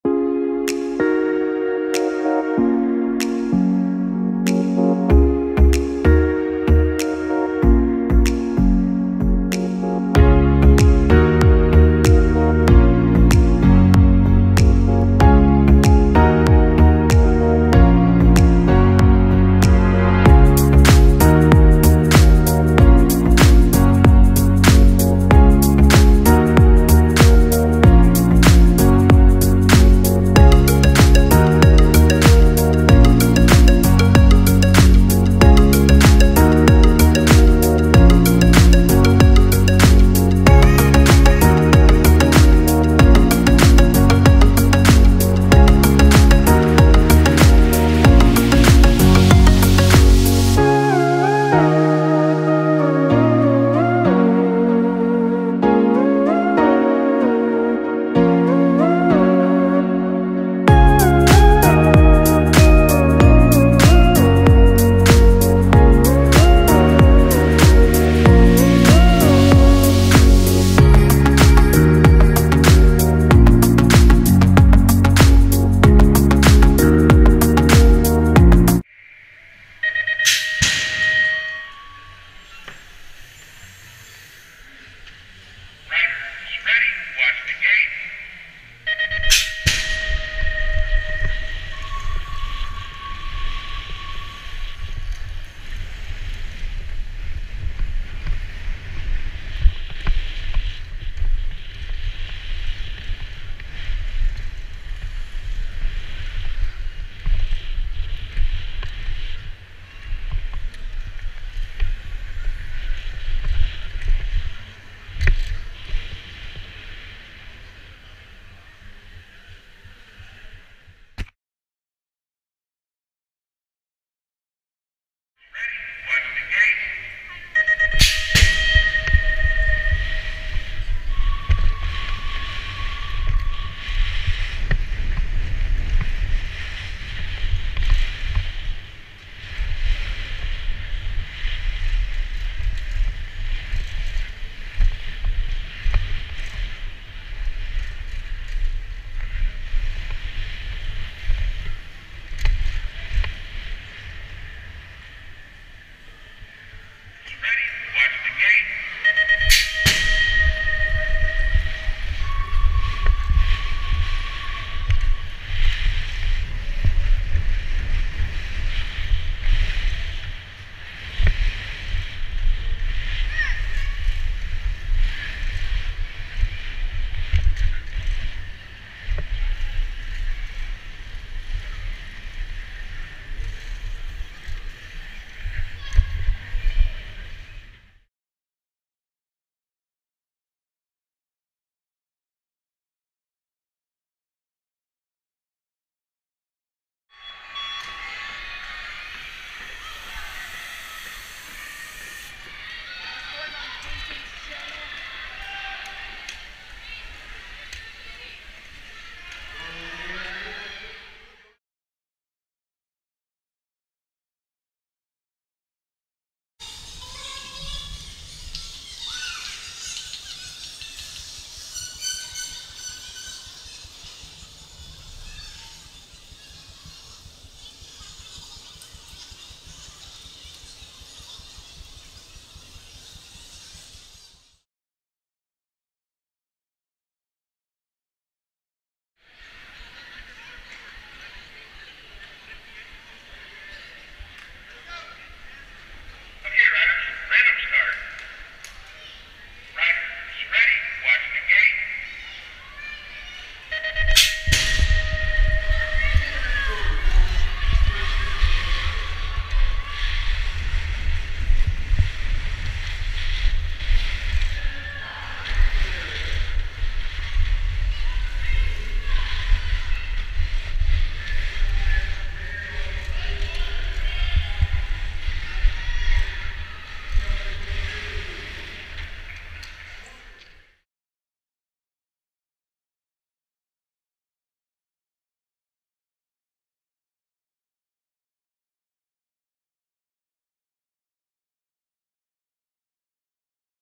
Grippen Park BMX - Double Point Weekend (With GoPro POV)
Grippen Park BMX - Double Point Weekend
With GoPro footage